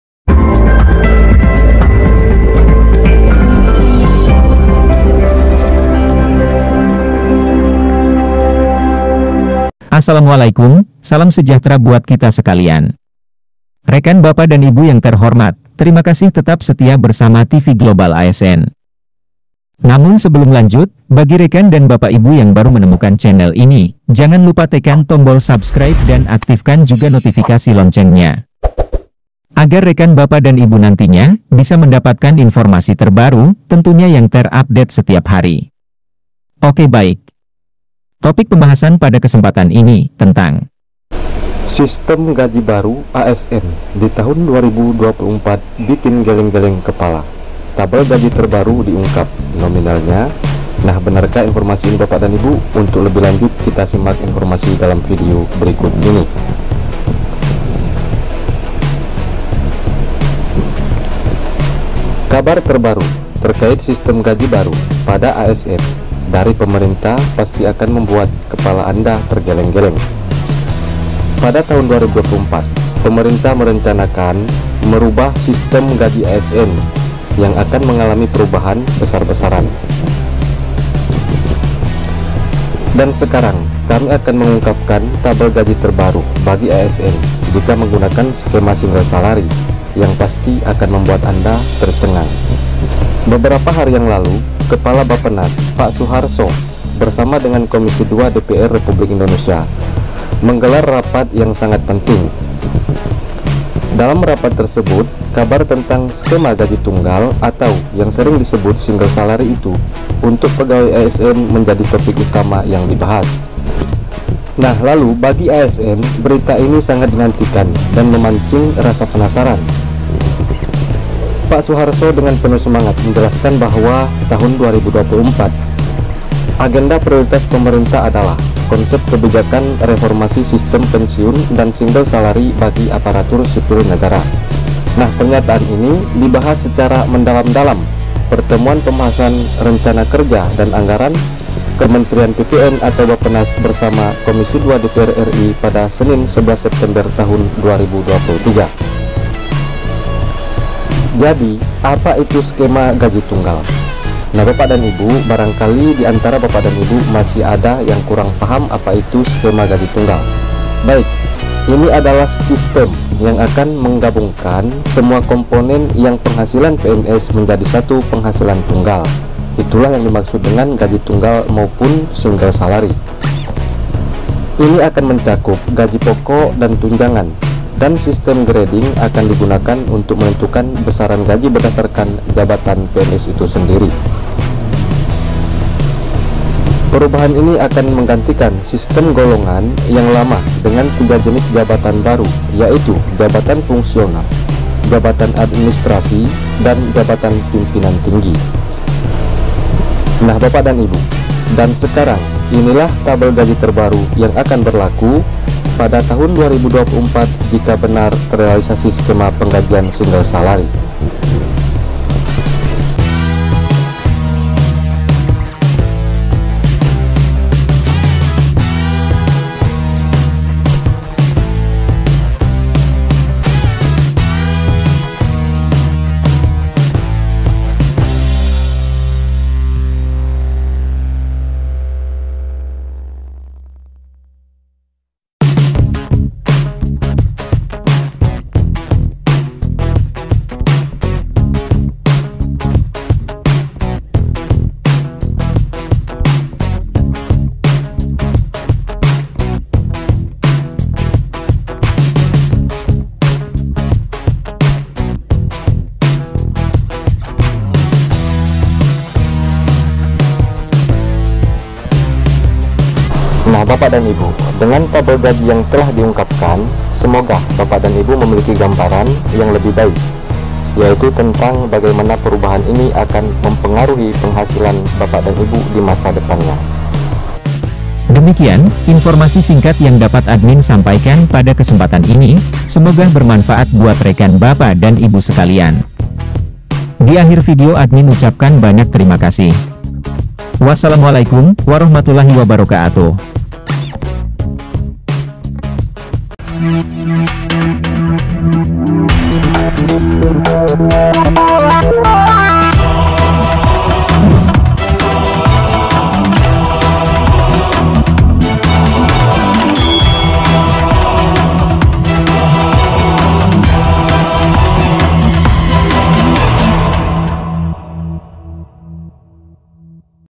SIAP - SIAP‼️SISTEM GAJI BARU ASN DI 2024, TABEL GAJI SINGLE SALARY TERBARU DIUNGKAP!
SISTEM GAJI BARU ASN DI 2024 BIKIN GELENG-GELENG KEPALA, TABEL GAJI TERBARU DIUNGKAP!

Kabar terbaru terkait sistem gaji baru pada ASN dari pemerintah pasti akan membuat kepala Anda tergeleng-geleng!
Pada tahun 2024, pemerintah merencanakan merubah sistem gaji ASN yang akan mengalami perubahan besar-besaran.

Dan sekarang, kami akan mengungkapkan tabel gaji terbaru bagi ASN jika menggunakan skema single salary, yang pasti akan membuat Anda tercengang!